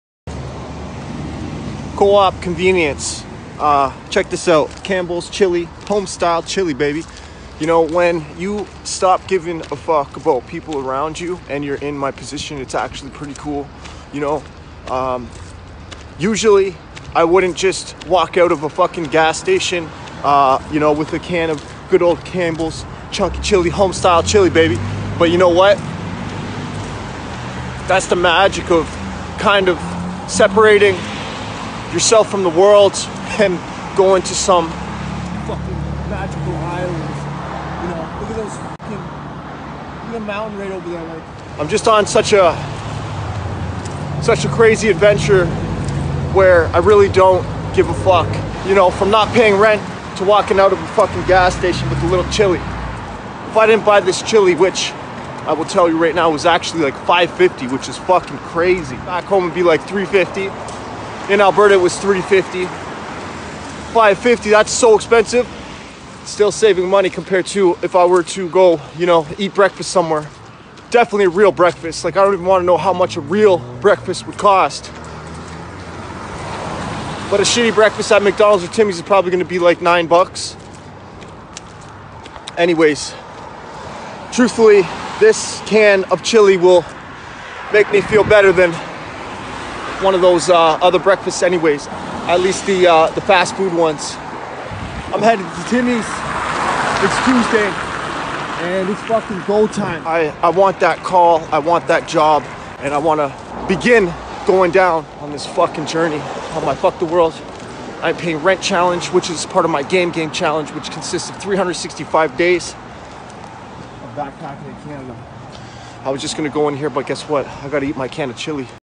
Cranking Back Cans Of Chilli These Days
Going and grabbing a can of good old Campbells Chilli Chunky Homestyle Chilli Chilli from the gas station is nothing big. But when it becomes a thing, and you just walk down the street with it and crank it back like a beer. It’s kinda got a FTW vibe to it. You know. The journey’s real. Here I am cranking back cans of food on Van Isle. Money tight as hell. 65L backpack still on. I’m just out here urban camping longterm hoping for a job call.

I hope you enjoy this video and follow along my JOURNEY!